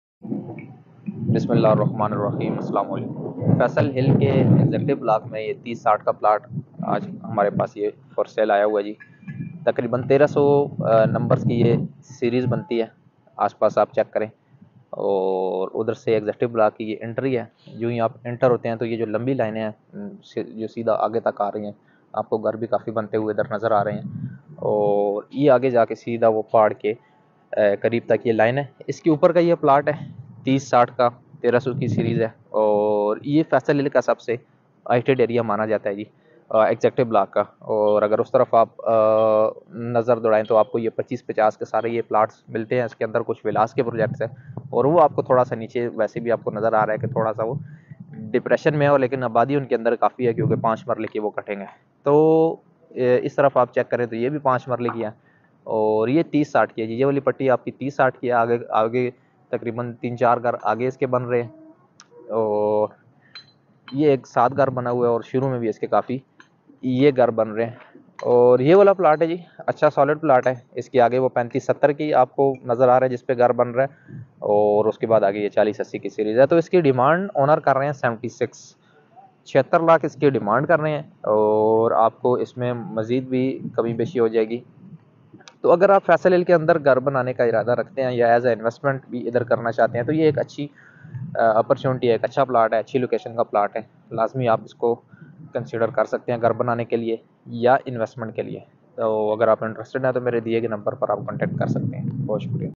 Faisal Hills Executive Block | 7 Marla Plot for sale in executive block
Faisal Hills Islamabad/Taxila, Executive Block is developing at a fast pace.

The update of faisal Hills Taxila/Islamabad Exective Block:
5,7Marla Plot of exective block is on sale